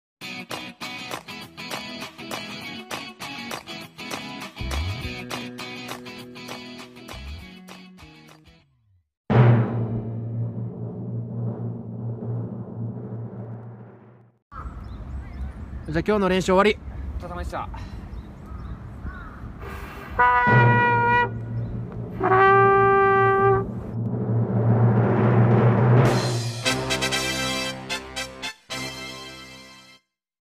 【毎日吹奏楽部あるある動画４２日目】《合奏終わってすぐに音出しする奴ほど》
吹奏楽芸人が吹奏楽部あるあるを毎日発信していきます！！！！！
皆さん是非ともお力添えをお願い致します＼(^o^)／